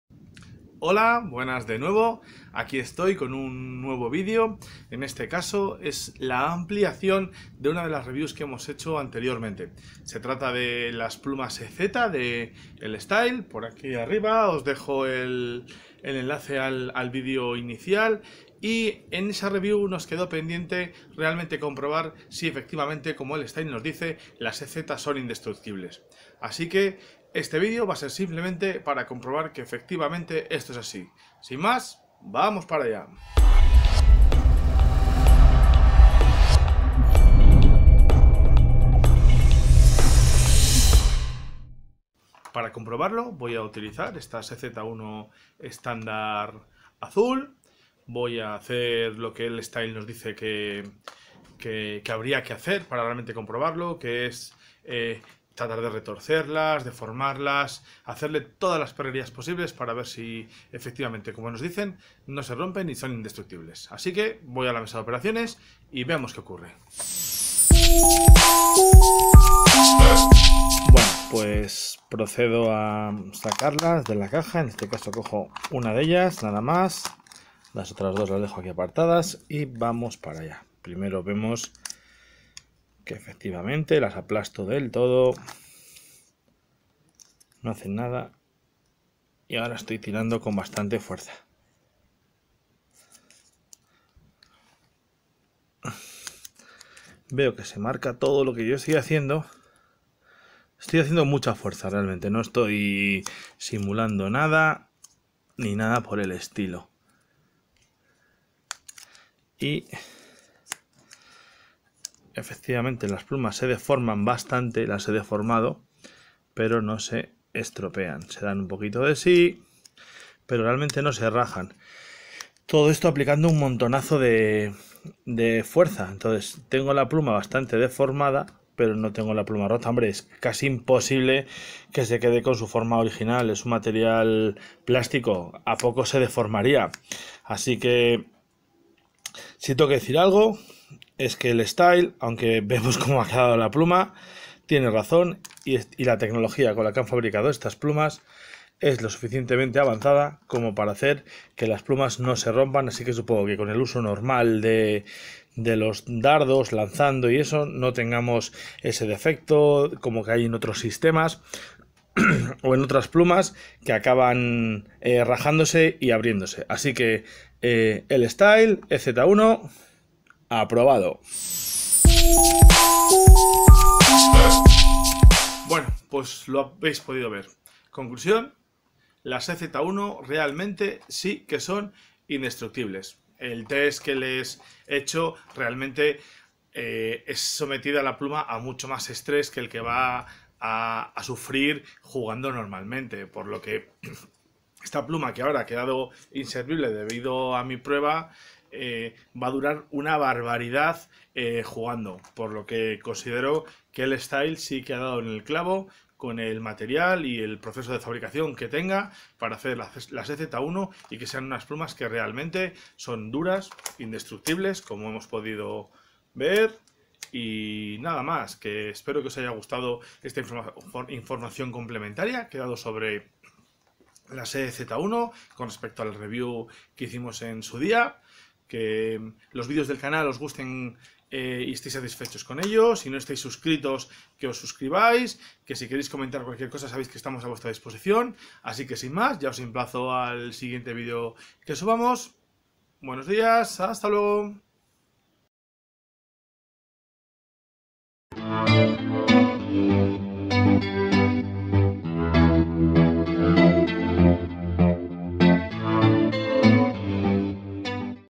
L - STYLE EZ, ¿se rompen las plumas para dardos L-Style EZ?
L-STYLE, ha fabricado recientemente las plumas para dardos Champagne EZ. ¡No se rompen!

En Comprardardos, hicimos una encuesta a través de nuestro Facebook Un 25% de las respuestas fueron que las plumas si se rompen.

Puedes comprobar en nuestro vídeo, que NO SE ROMPEN.

Y recuerda...
Si no quieres perderte la mejor información sobre dardos en español, ¿a qué esperas?

¡Forma parte de la familia Comprardardos!